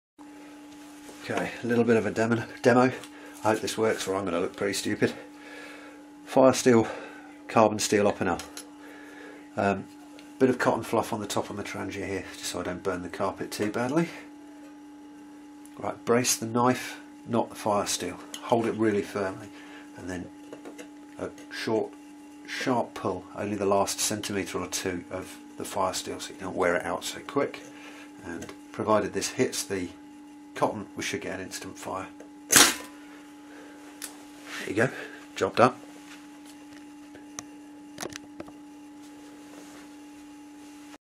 firesteel
Quick firestarting with a ferro rod and some cotton wool.